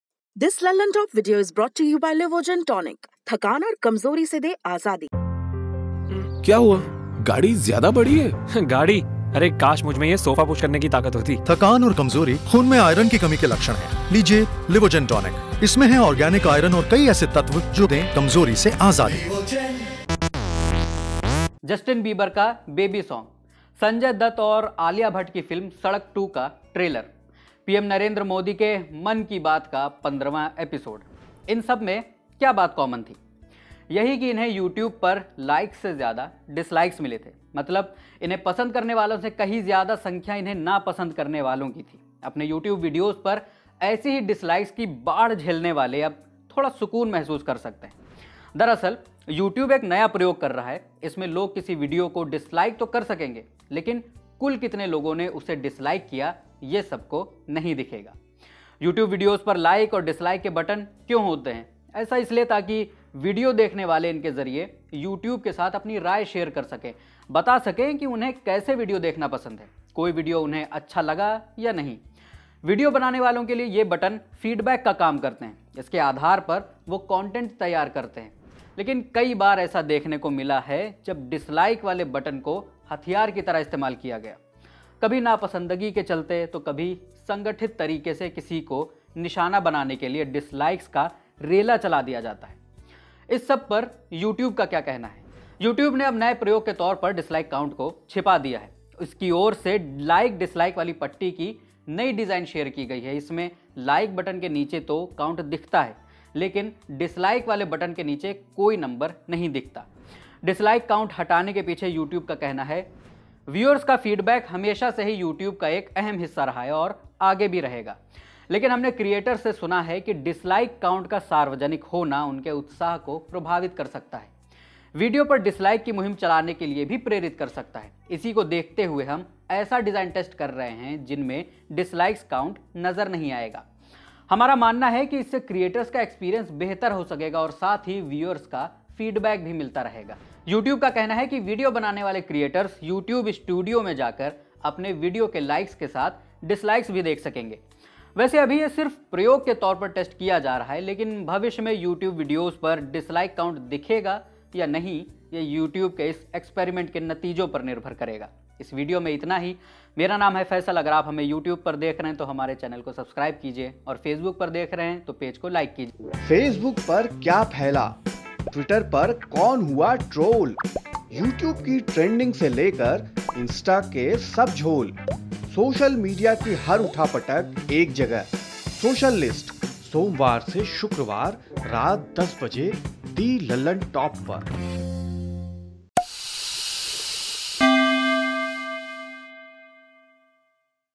YouTube अपने dislike count को छिपाने का प्लान बना रहा है; जानिए क्या बदलेगा
थकान और कमज़ोरी से दे आज़ादी

YouTube is undergoing an experiment to test a new design where viewers can like or dislike a video but cannot see the dislike count. The creator will still see the like and dislike count in the YouTube studio for user feedback but the dislike count will be not be visible to the public. Watch the video to know more.

Edited By:Mohan